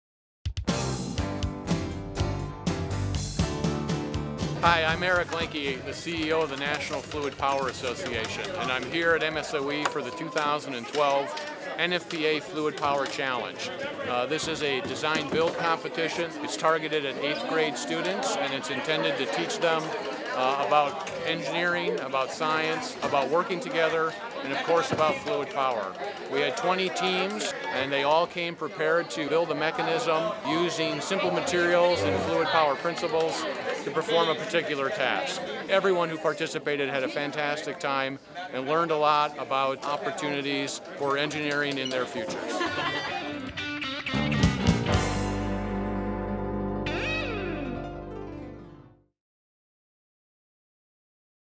2012 Fluid Power Challenge
Twenty-one teams of eighth grade students were given the assignment of designing and constructing a fluid power mechanism to perform a defined task. After working for four weeks, the teams came together to compete against each other in a two-minute competition. Engineers from area companies served as judges.